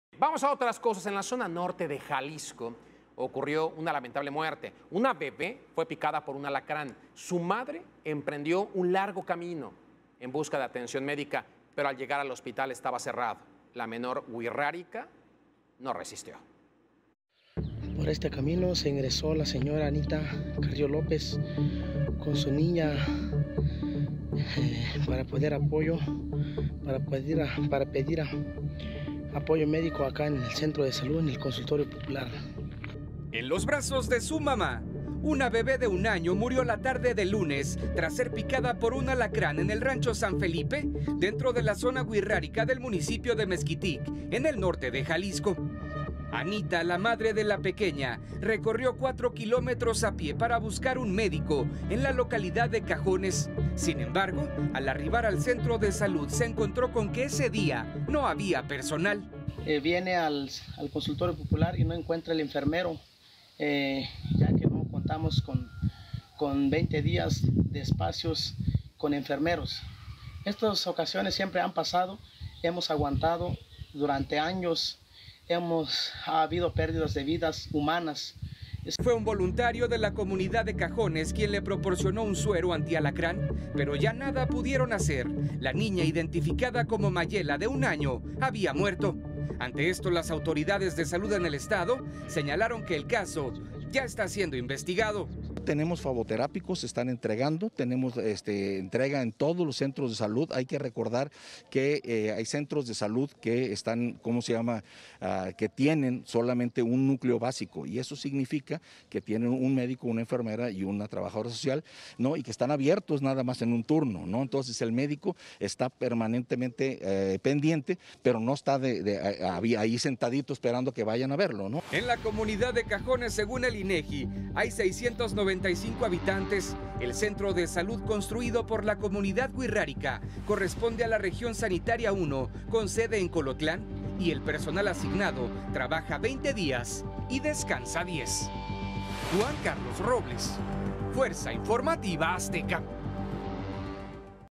En la zona norte de Jalisco ocurrió una lamentable muerte, una bebé fue picada por un alacrán
En la zona norte de Jalisco ocurrió una lamentable muerte, una bebé fue picada por un alacrán, su madre emprendió un largo camino en busca de atención médica.
Con información de Juan Carlos Robles